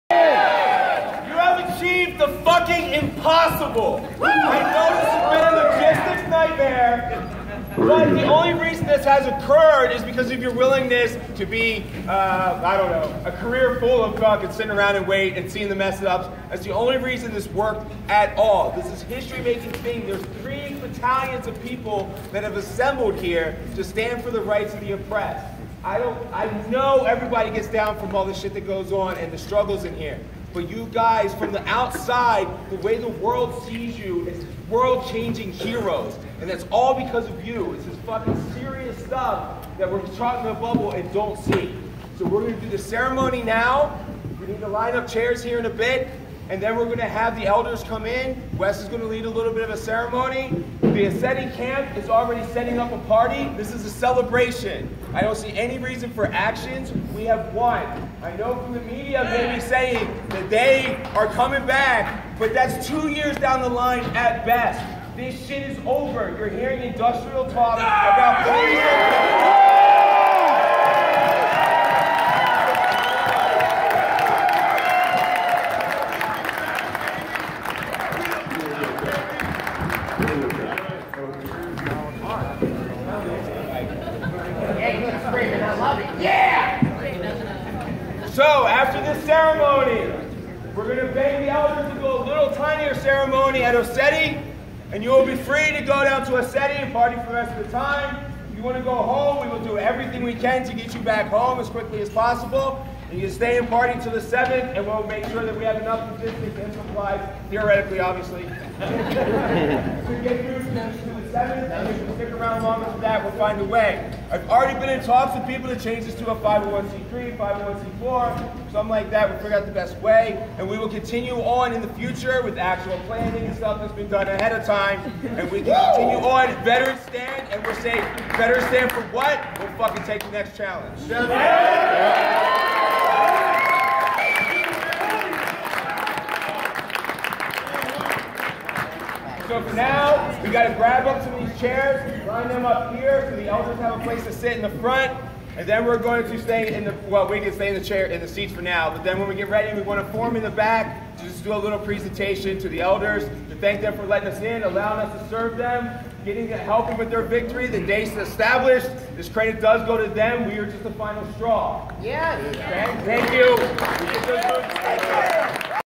NoDAPL Veterans Stand Michael Wood Jr. Pre-Ceremony Pep Talk
This occurred at the Prarie Knights Casino and Resort Pavillion yesterday morning (5 DEC 2016). Unfortunately, ETP said they will continue drilling and pay the $50k/day fine to contunue the pipeline even though USACE said no drilling until an environmental impact statement (EIS) is complete.